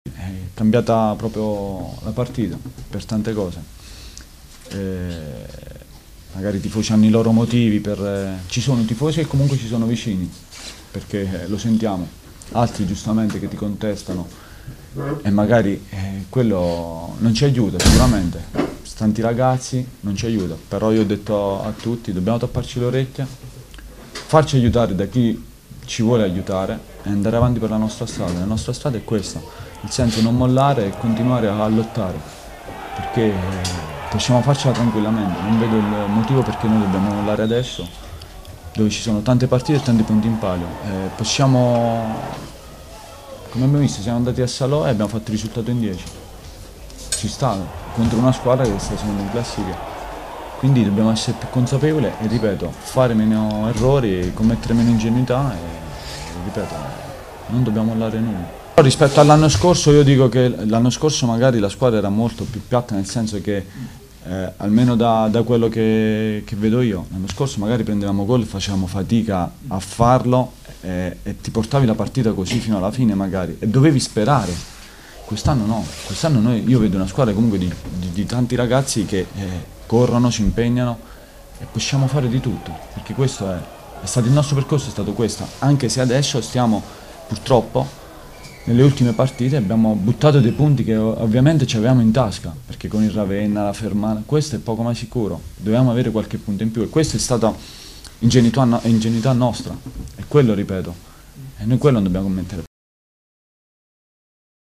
Teramo in crisi, Speranza: “Non molliamo”
Il maltempo condiziona anche il lavoro del Teramo che ha ripreso gli allenamenti nel primo pomeriggio nella struttura sportiva “Interamnia”. Domani doppia seduta. Intanto in vista della trasferta proibitiva contro la capolista Padova di domenica prossima il morale in casa biancorossa è sotto i tacchi a seguito della debacle casalinga col Bassano. Ma il capitano Ivan Speranza non ci sta e confida in una reazione della squadra